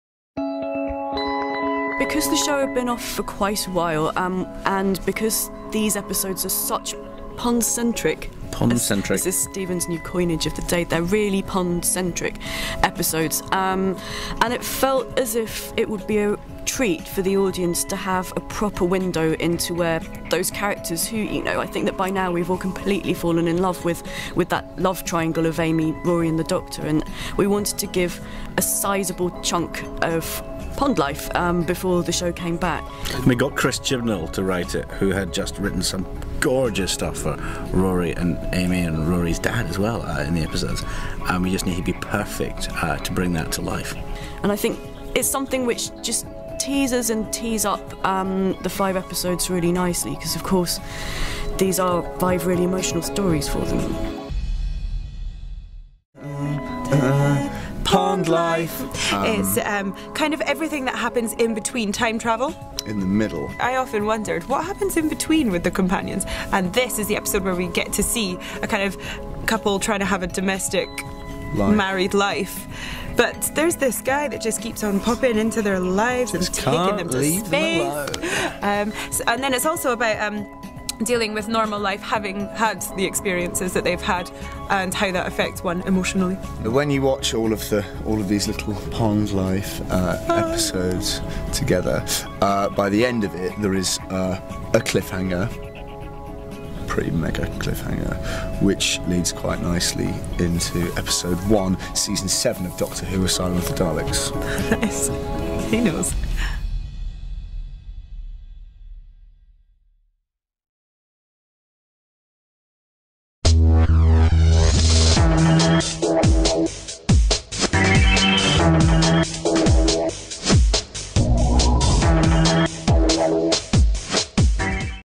Doctor Who - The 'Pond Life' Interviews Together
A chance to see the Doctor Who 'Pond Life' Interviews Together, Steven Moffat, Caro Skinner, Karen Gillan and Arthur Darvill talk about the mini-sode prequels